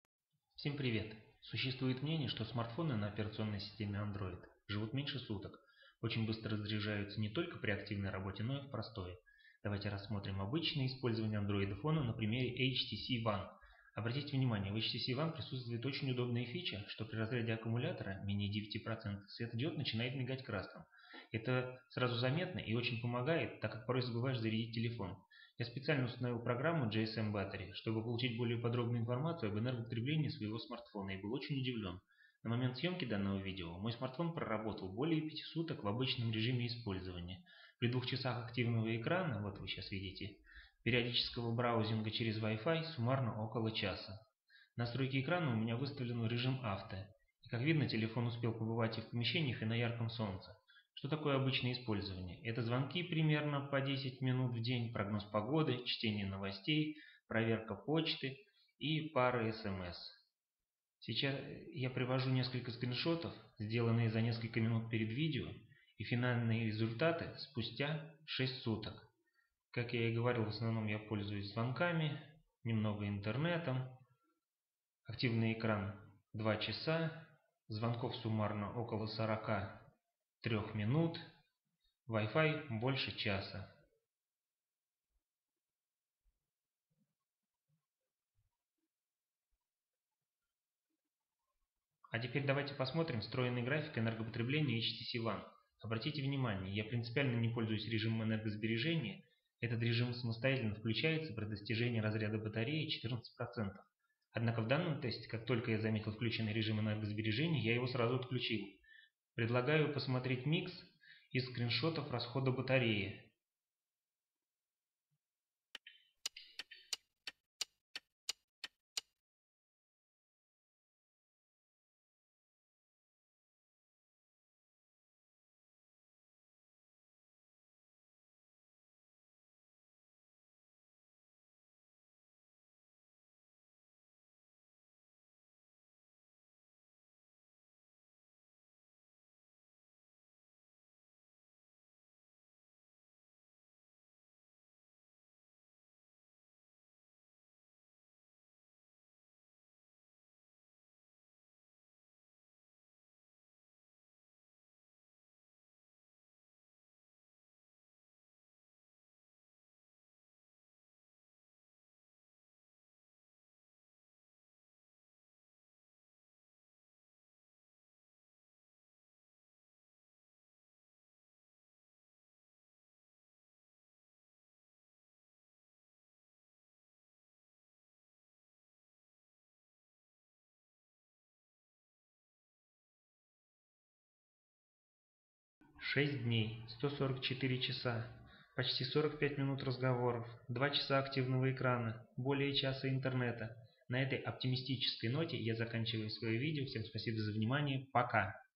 HTC ONE: Миф о "жрущем" Android'е
Существует мнение, что смартфоны на операционной системе Андроид живут меньше суток, очень быстро разряжаются не только при активной работе, но и в простое.
Давайте рассмотрим использование android-о-фона, на примере HTC ONE.

Нормальные люди (не мы - гики) несколько раз в сутки принимают звонки и сами звонят, пару раз заходят в интернет (браузинг, почта, скайп), отправляют и принимают СМС.
Нормальные люди не играют в игрушки, не сидят в социалках, не "инстаграмятся" каждые десять минут.